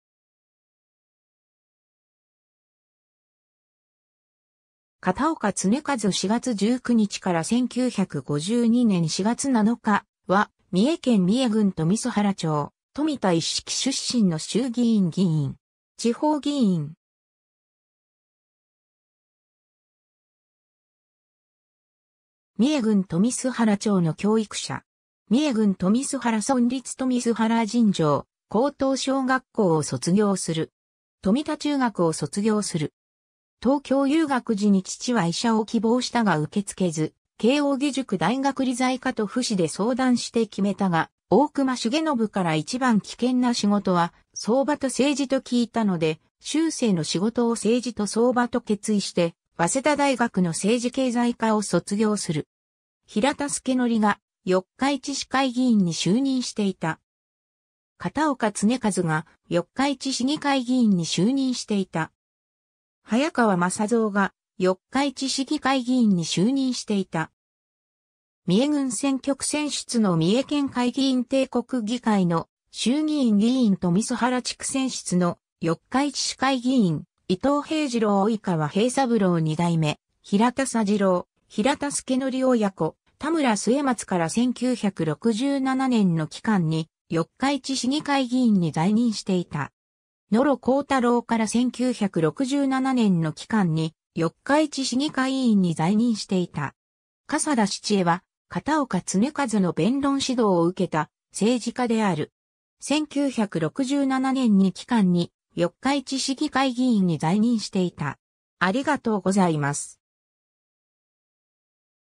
片岡恒一
三重県選出の衆議院議員_(帝国議会)
片岡 恒一（かたおか つねかず、1896年（明治29年）4月19日 - 1952年（昭和27年）4月7日）は、三重県三重郡富洲原町（現在の四日市市富洲原地区）富田一色出身の衆議院議員。
地方議員（三重県会議員・四日市市会議員）。
三重郡富洲原町の教育者。
 三重郡富洲原村立富洲原尋常高等小学校（現在の四日市市立富洲原小学校）を卒業する。
 富田中学（現在の三重県立四日市高等学校）を卒業する。
 東京遊学時に父は医者を希望したが受け付けず、慶應義塾大学理財科と父子で相談して決めたが、大隈重信から一番危険な仕事は相場と政治と聞いたので、終生の仕事を政治と相場と決意して早稲田大学の政治経済科（政治経済学部）を卒業する。
 平田佐矩が四日市市会議員に就任していた。
（富田一色出身の平田紡績社長と富洲原町会議員から昭和16年から昭和22年まで四日市市会議員に就任した。戦後助役となり、昭和34年から四日市市長となる）。
 片岡恒一が四日市市議会議員に就任していた。
（富田一色出身の三重県会議員と帝国議会の衆議院議員から昭和17年から昭和22年に四日市市会議員となる）。
 早川政蔵が四日市市議会議員に就任していた。
（天ヶ須賀出身の富洲原町会議員から昭和16年から昭和30年まで四日市市議会議員となる）。
 三重郡選挙区選出の三重県会議員 帝国議会の衆議院議員 富洲原地区選出の四日市市会議員 伊藤平治郎 生川平三郎 2代目平田佐次郎・平田佐矩親子 田村末松（1955年（昭和30年） - 1967年（昭和42年）の期間に四日市市議会議員に在任していた）。
 野呂幸太郎（1955年（昭和30年） - 1967年（昭和42年）の期間に四日市市議会員に在任していた）。
 笠田七衛は片岡恒一の弁論指導を受けた政治家である。
（富田一色出身。1959年（昭和34年） - 1967年（昭和42年）に期間に四日市市議会議員に在任していた）。